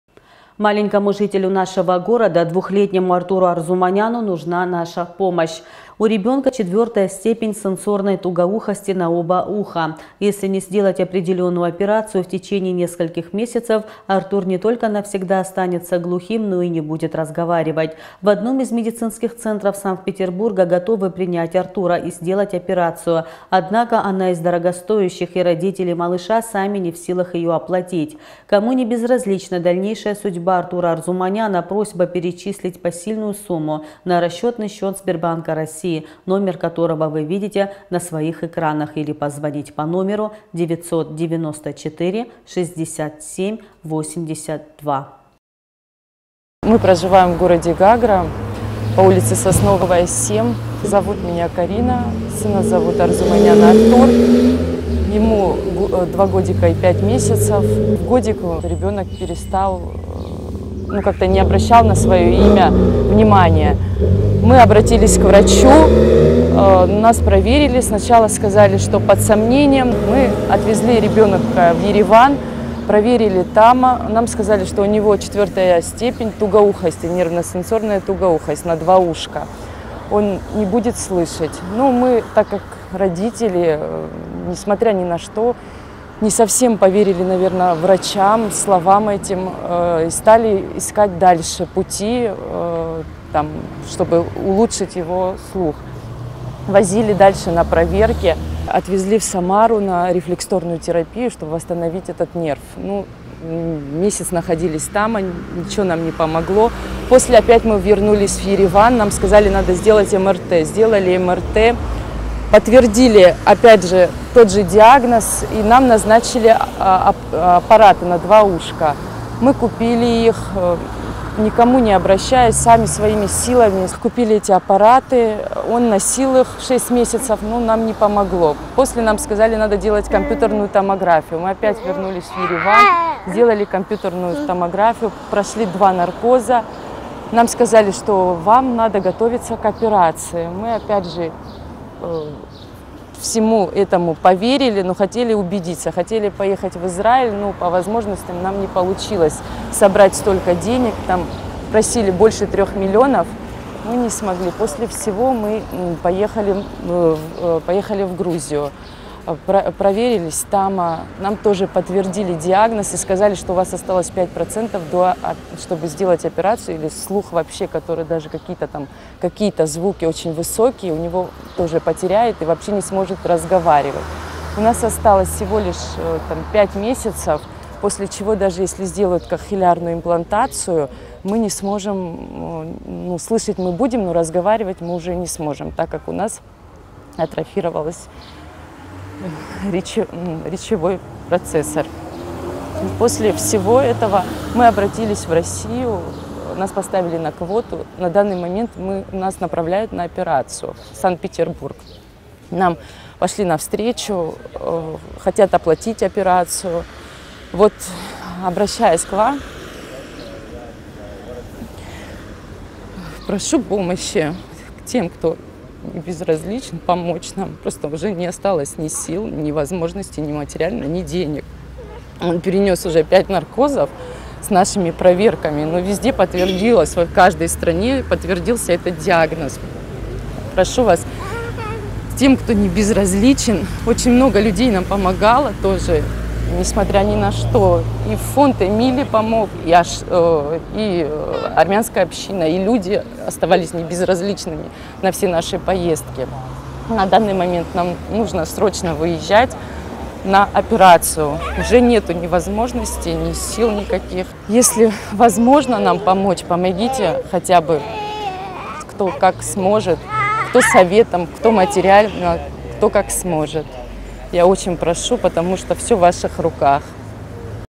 Поможем маленькому Артуру вместе
Маленькому жителю нашего города, двухлетнему Артуру Арзуманяну нужна наша помощь. У ребенка четвертая степень сенсорной тугоухости на оба уха.